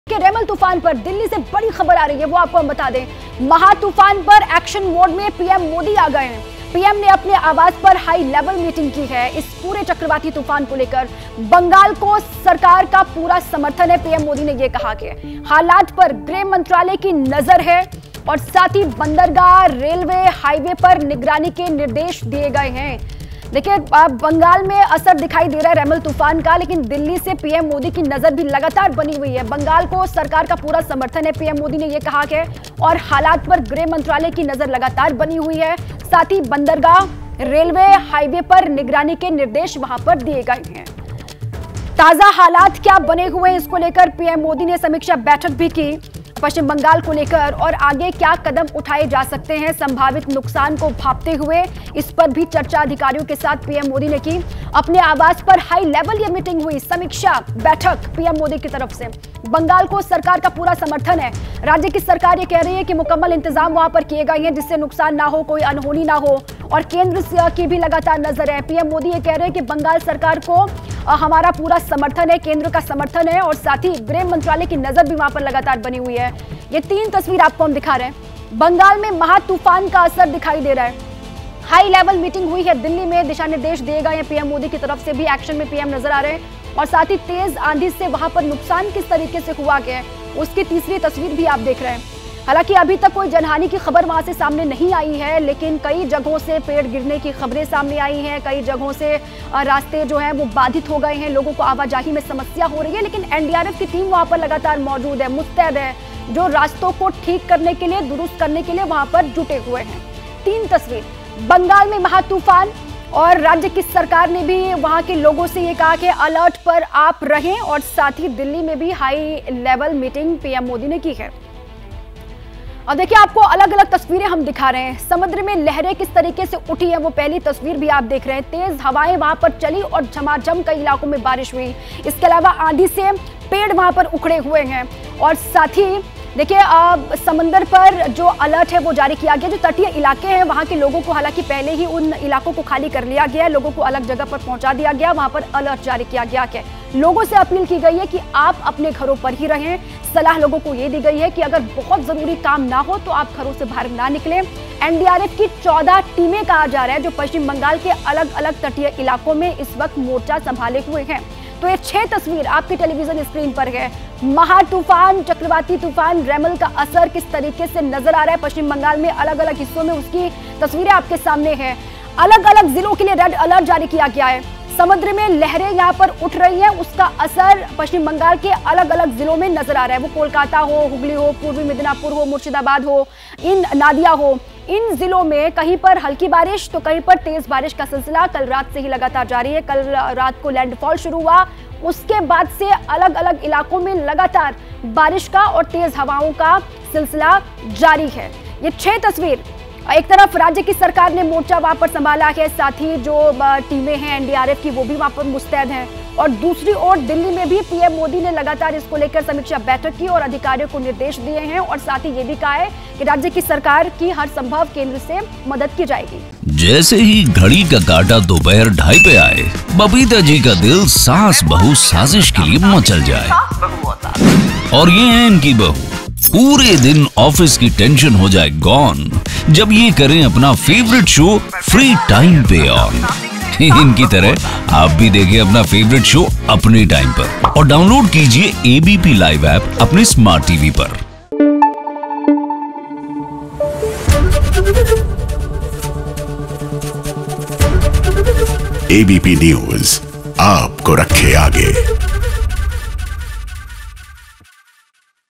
Breaking News: महातूफान को लेकर एक्शन मोड में PM Modi | Cyclone Remal | ABP News
ABP News: सावधान...आ गया महातूफान...बंगाल की खाड़ी से उठा रेमल तूफान पश्चिम बंगाल पहुंच चुका है...इस बीच रेमल तूफान पर दिल्ली से बड़ी खबर आ रही है- महातूफान पर एक्शन मोड में पीएम मोदी आ गए हैं, पीएम ने अपने आवास पर हाईलेवल मीटिंग की है, बंगाल को सरकार पूरा समर्थन, हालात पर गृह मंत्रालय की नजर, बंदरगाह, रेलवे, हाईवे पर निगरानी के निर्देश. रेमल तूफान ने दस्तक के साथ ही कहर बरपाना शुरू कर दिया है..बंगाल समेत कई राज्यों में अलर्ट है...लेकिन बंगाल इस वक्त ज्यादा प्रभावित है.